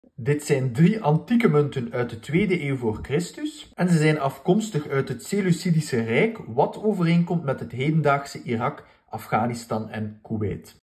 3 antieke munten die 2200jaar oud zijn
Prachtige munten uit het Seleucidische rijk